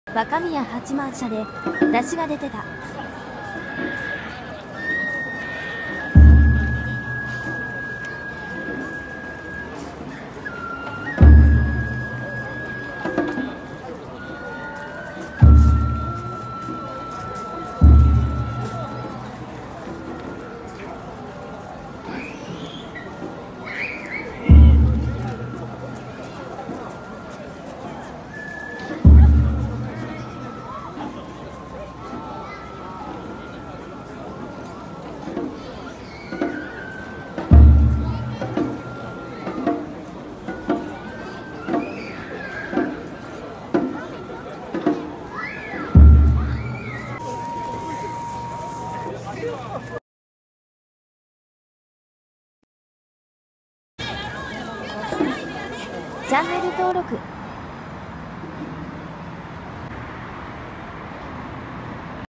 【愛知県】若宮八幡社 の山車。名古屋の山車は珍しい。
山車は、レア。3年ぶりか？テレビで放送してた。